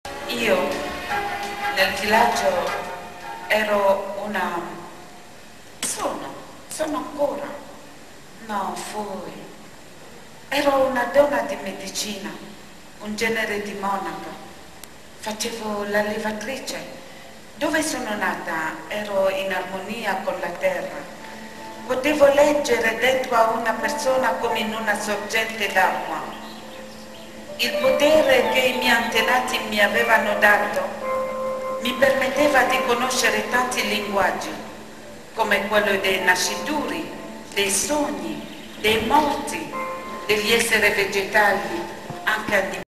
Settimana della lingua italiana nel mondo
Performance di:
Felicitè Mbezele
autrice di Kanteros. Unafricana a Roma (Armando 2006)